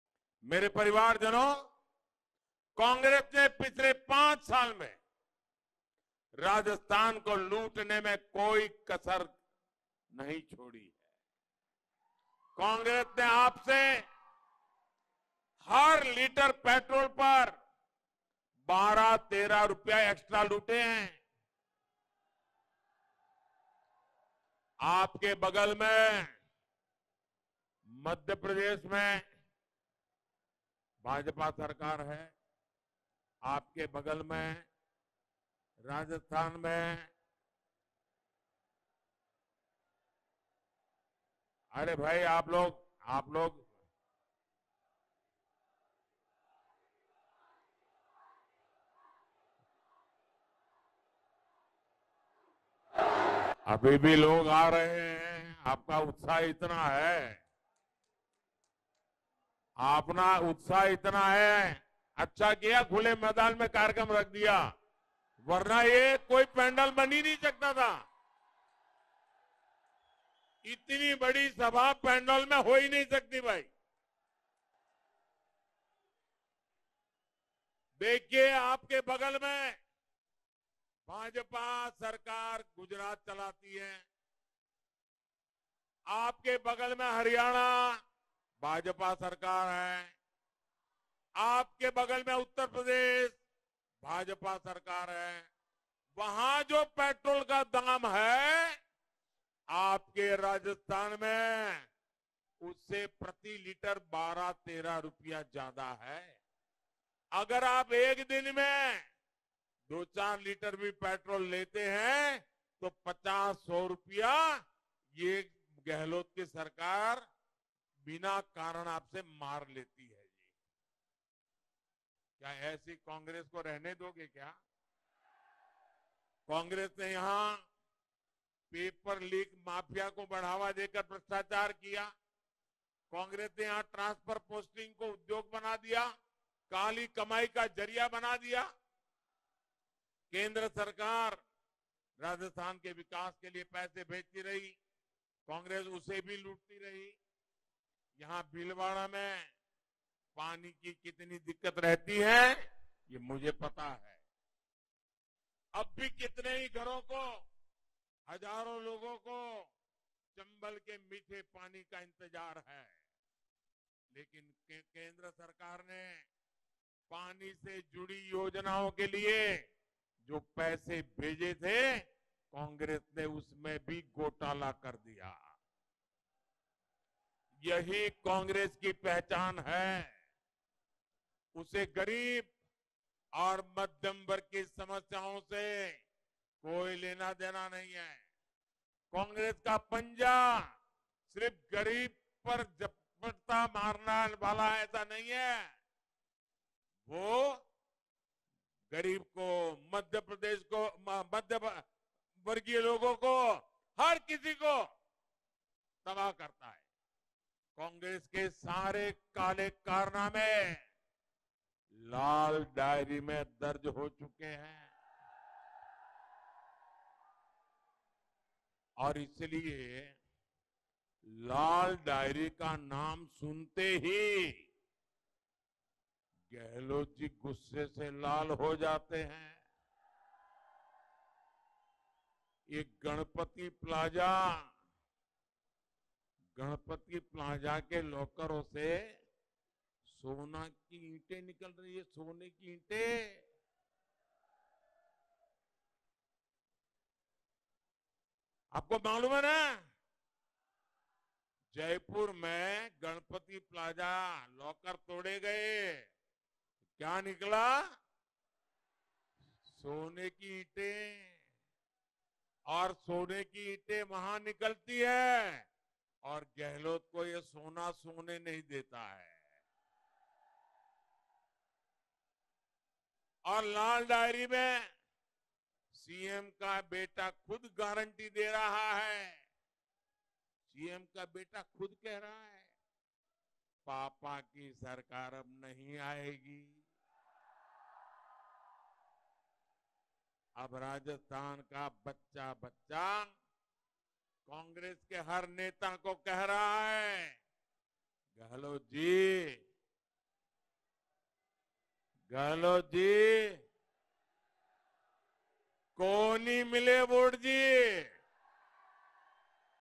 लाल डायरी का नाम सुनते ही Rajasthan के मुख्यमंत्री क्यों हो जाते हैं गुस्से से लाल?
Prime Minister Narendra Modi, addressing an election rally in Kotri, Rajasthan, said that Congress didn’t leave any stone unturned in looting Rajasthan in the last five years. From increasing prices of petrol and diesel to promoting the paper leak mafia and making transfer postings a source of black money, Congress has looted people on all fronts. PM Modi also accused Congress of corruption in central government water related schemes and said that I understand the water scarcity in this region and we had sent funds to provide water to every household but, the Congress government scammed that money too depriving my brothers and sisters of access to clean drinking water.